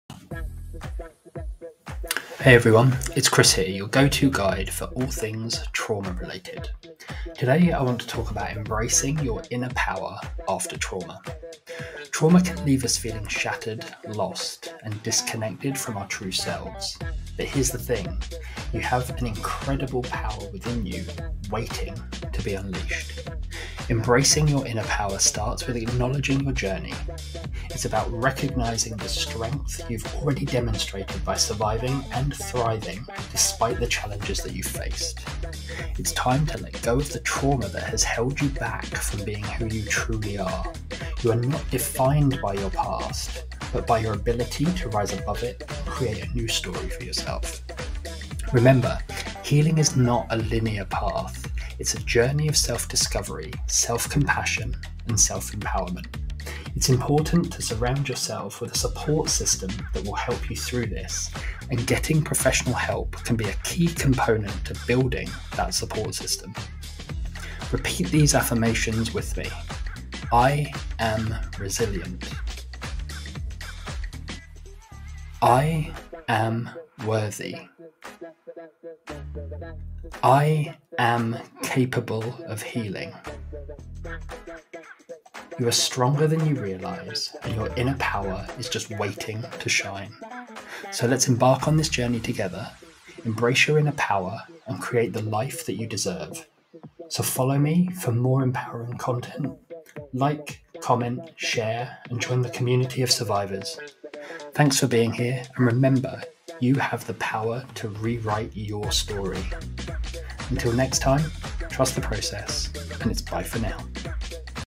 Embracing Your Inner Power: A Journey of Healing and Triumph
Welcome to a transformational experience in "Embracing Your Inner Power - A Journey of Healing and Triumph." In this uplifting video, we delve into the path of self-discovery, guiding you towards healing and triumph over life's obstacles. Discover the immense power within you and witness the incredible journey of those who turned their struggles into strengths. Get ready to embrace your true self and unleash the potential that resides in your heart.

Embark on a soul-stirring quest of self-realization with "Embracing Your Inner Power - A Journey of Healing and Triumph." Weaving together stories of courage, resilience, and transformation, this video brings you the inspiration you need to overcome life's adversities. Uncover the secrets of inner healing and embrace the power that dwells within you. Get ready to be moved, motivated, and uplifted as we embark on this extraordinary journey together.